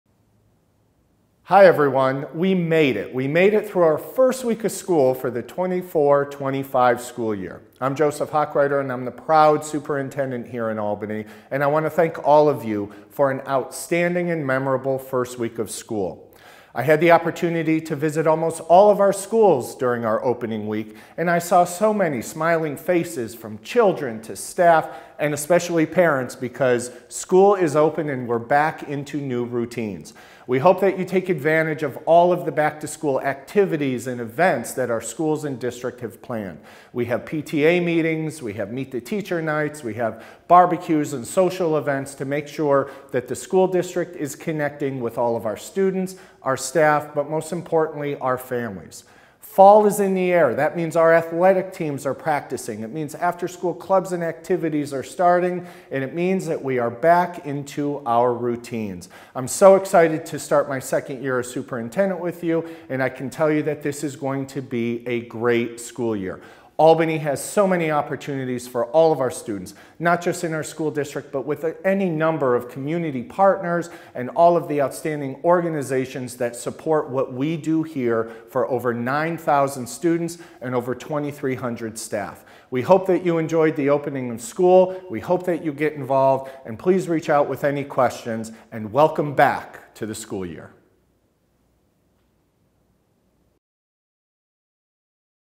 A Great First Week!
A message of positivity and encouragement from Superintendent Hochreiter as we start our second full week of classes!